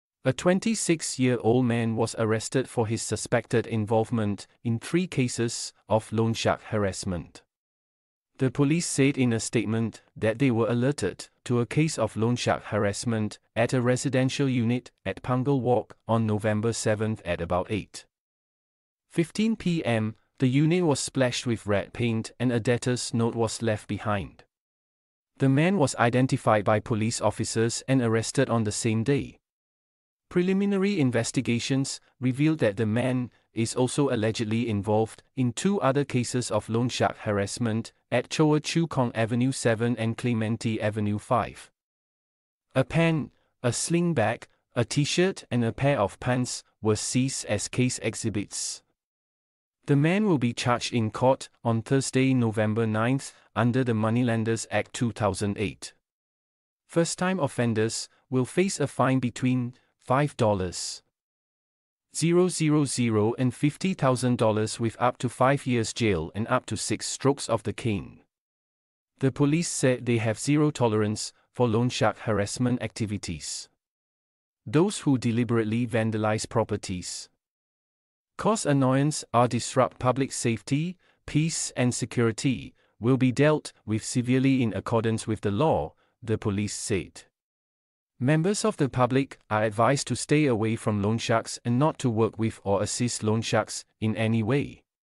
Man, 26, arrested for loanshark harassment at Punggol, Choa Chu Kang and Clementi
In this video, we will be discussing a recent incident involving a 26-year-old man who was arrested for loanshark harassment in three different locations - Punggol, Choa Chu Kang, and Clementi. Loansharking has become a growing concern in Singapore, with individuals resorting to illegal means to enforce loan repayments. We will delve into the details of this case, exploring the impact of loanshark harassment on both the victims and the community as a whole.
1. Loanshark harassment
2. Arrested
3. Man
4. Punggol
5. Choa Chu Kang
6. Clementi.

💗📌 Singapore